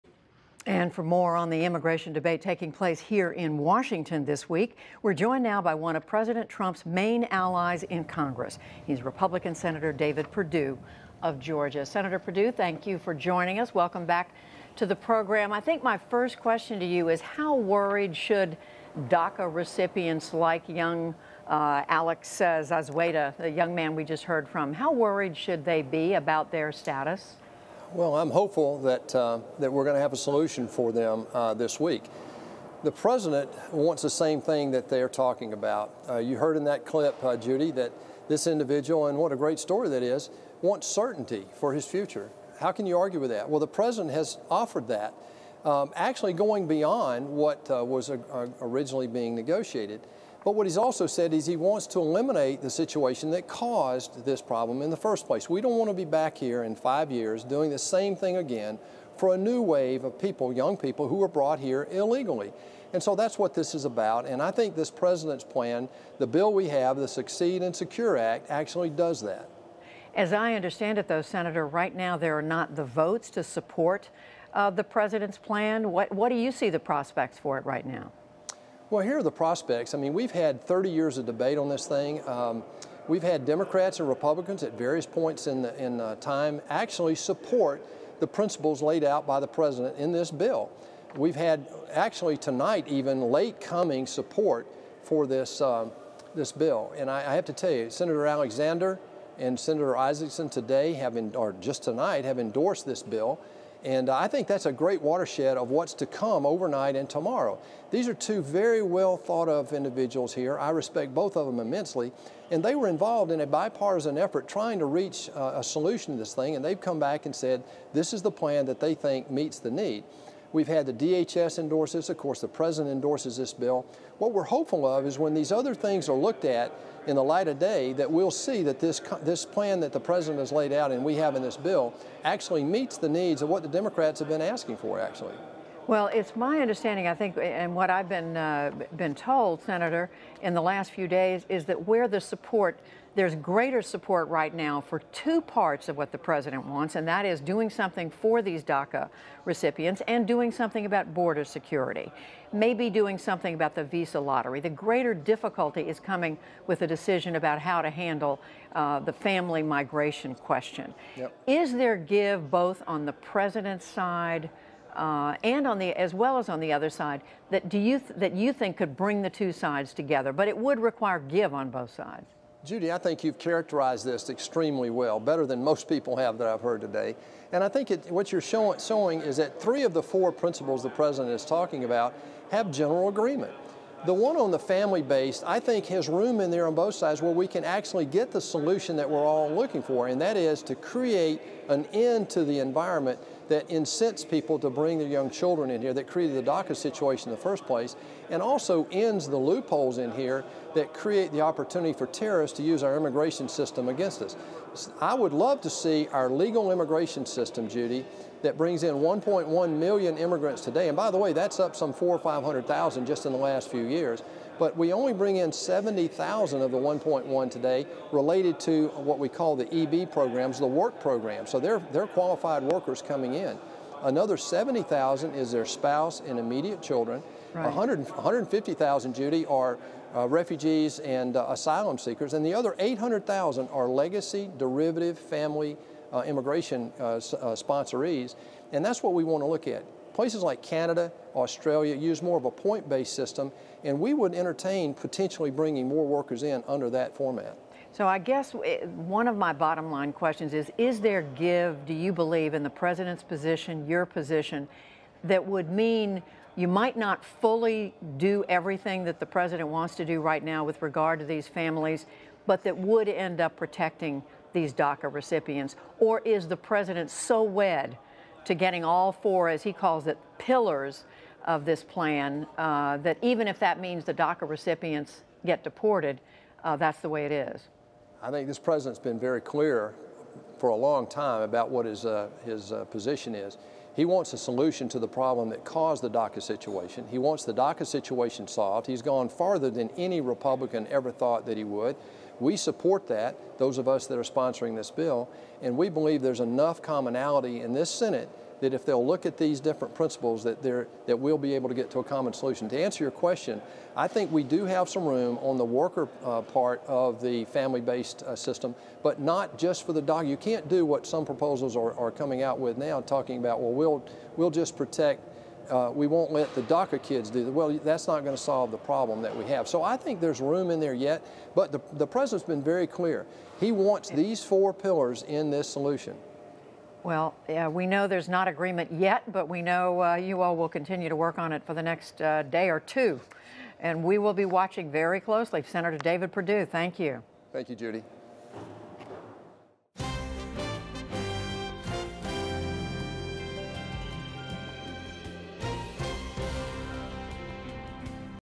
Sen. David Perdue: Trump wants all 4 'pillars' in immigration bill
Sen. David Perdue, R-Ga., says he is hopeful that Congress will support a solution for DACA recipients this week, while taking measures to prevent future waves of illegal immigration. Perdue joins Judy Woodruff to discuss his support for President Trump’s immigration plan and whether Republicans would give any ground in order to pass legislation to protect the “Dreamers.”